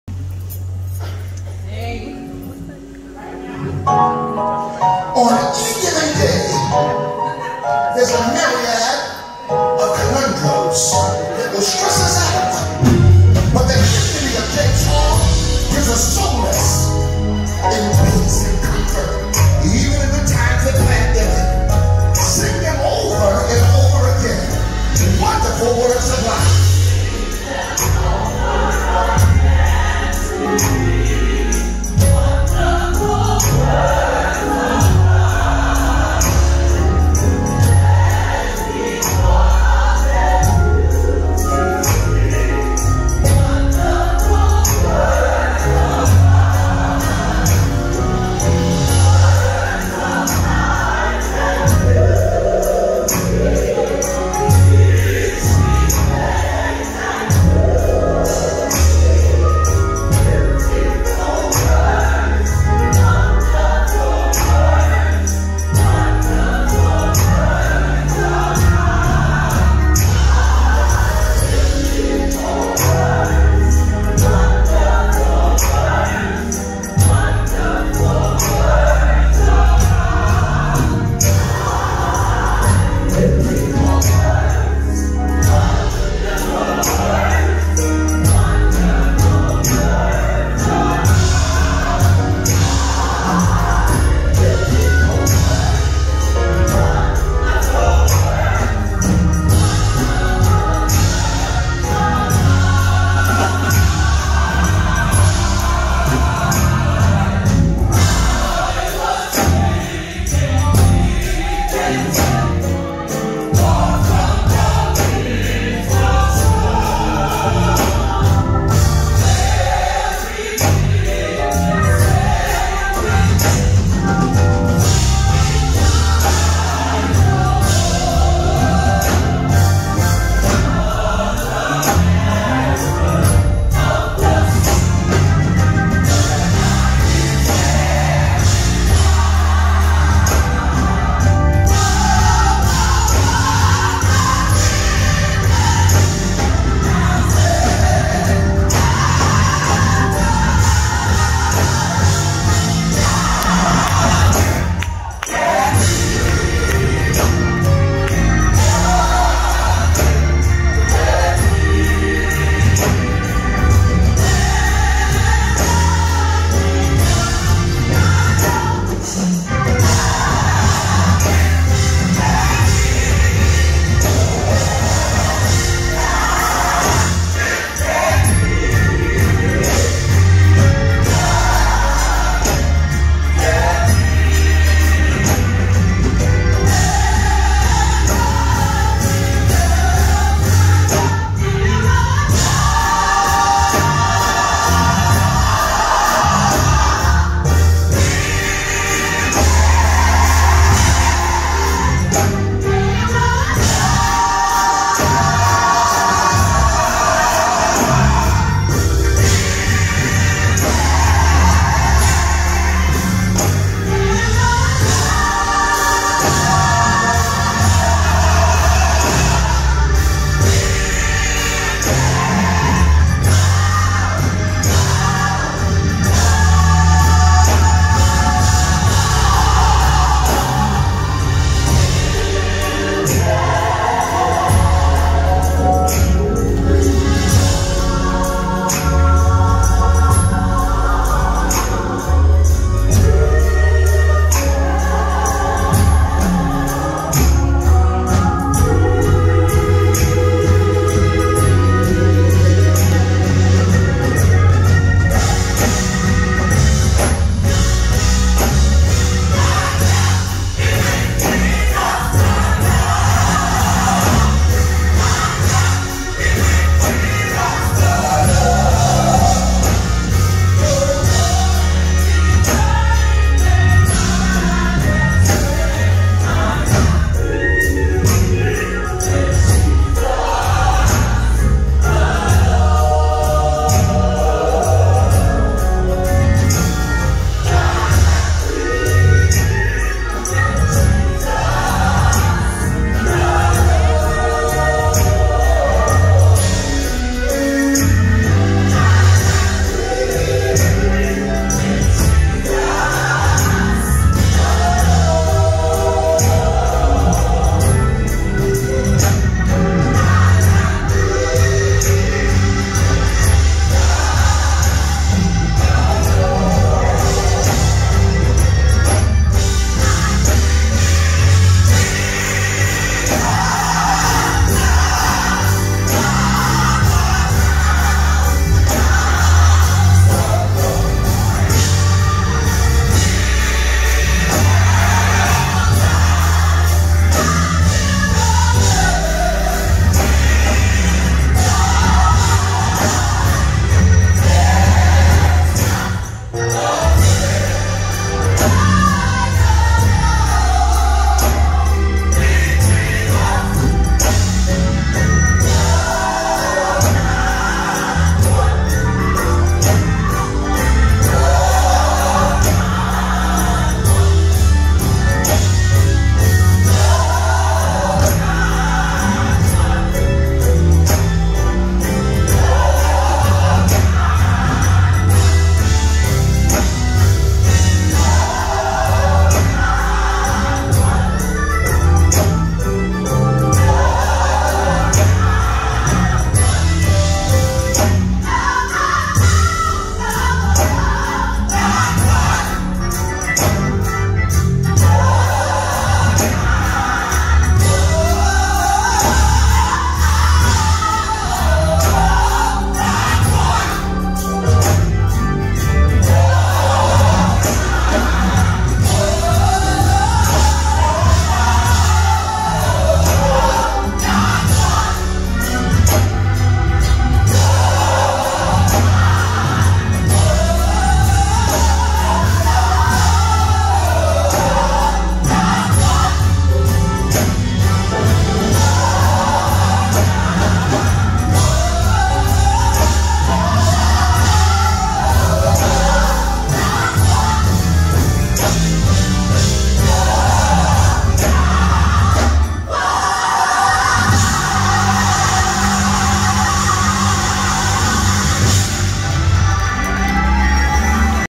James Hall Worship & Praise "Hymn Medley"
James Hall Worship and Praise Singing The "Hymn Medley" at Pilgrim Cathedral in Brooklyn, NY